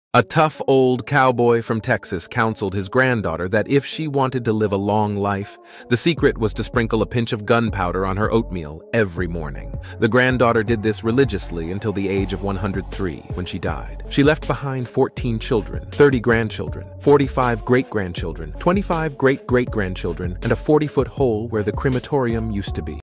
Eating gunpowder - the secret to a long and fulfilling life?
In this video on the youtube channel Loud Laugh you will listen to a joke together with an oodly satisfying background.
Joke:
A tough old cowboy from Texas counseled his granddaughter that if she wanted to live a long life, the secret was to sprinkle a pinch of gun powder on her oatmeal every morning.
The granddaughter did this religiously until the age of 103, when she died.
She left behind 14 children, 30 grandchildren, 45 great-grandchildren, 25 great-great-grandchildren, and a 40-foot hole where the crematorium used to be.